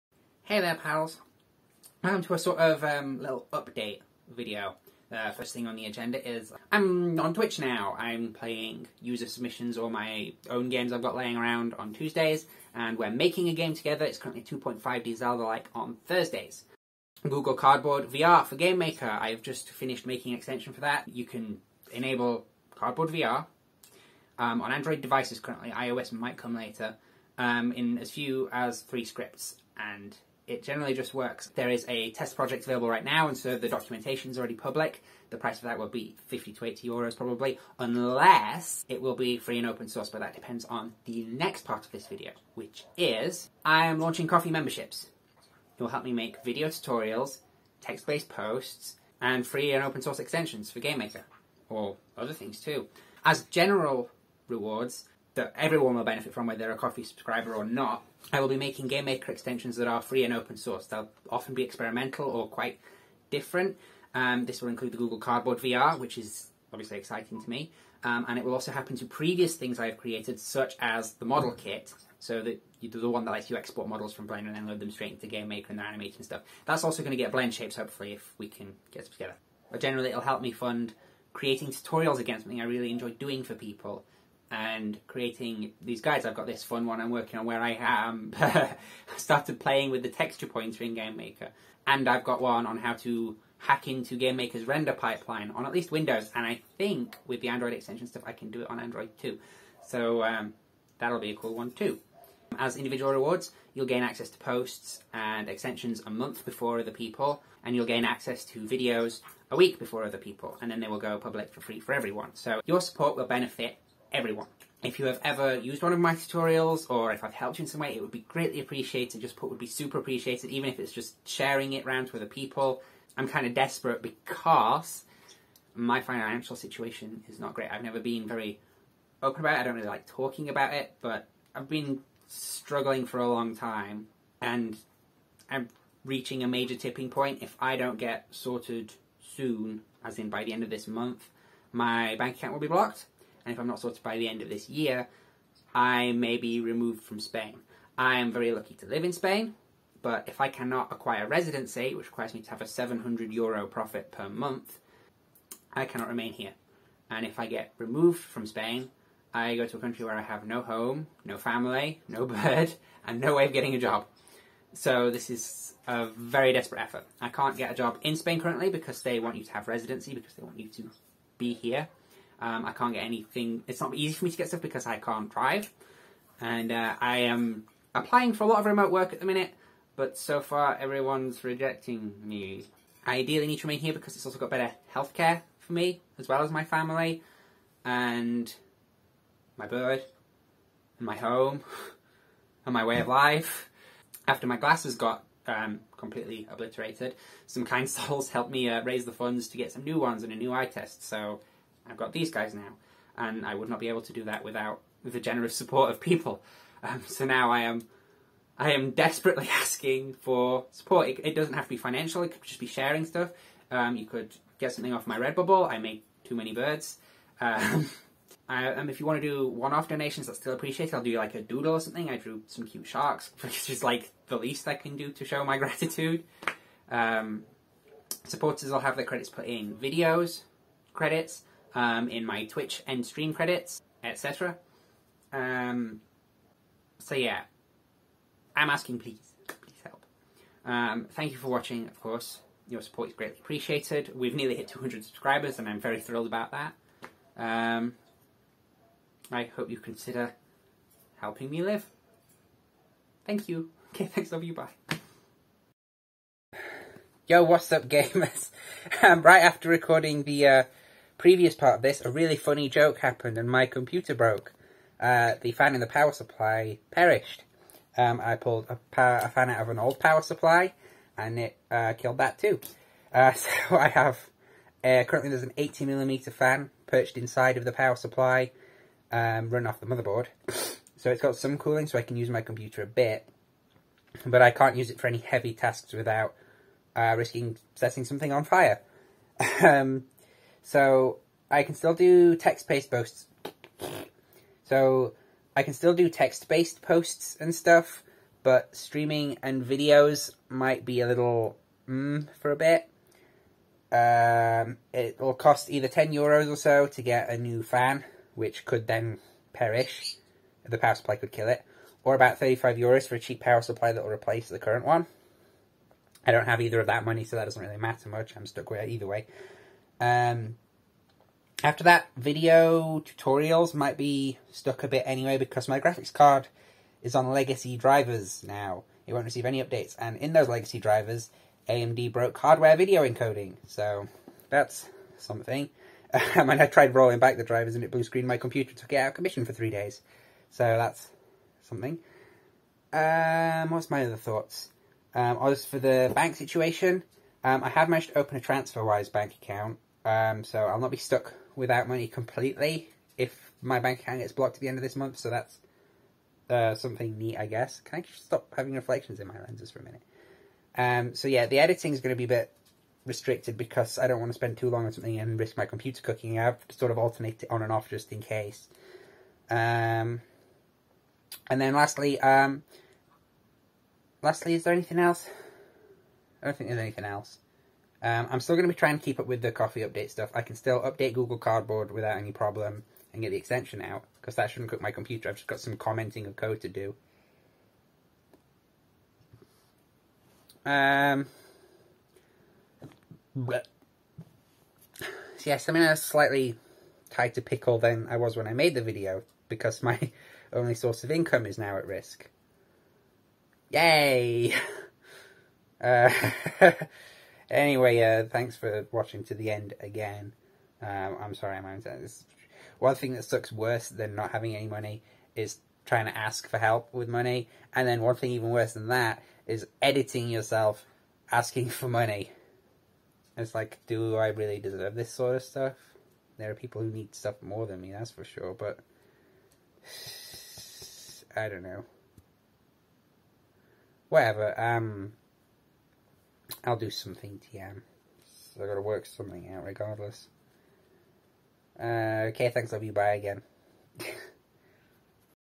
Update! Google Cardboard VR in GameMaker! Twitch Streams! Ko-Fi! (i'm poor lol)
Dang, asking for money super sucks, but heck, I'm kinda stuck. The cardboard thing I think is pretty cool though! I think I'll also make an up to date tutorial on making C++ extensions targeting Android soon too.

00:00 - Intro
00:06 - Twitch Announcement!
00:16 - Google Cardboard!
00:42 - Ko-Fi Memberships for Free Extensions!
04:15 - My computer broke after recording lol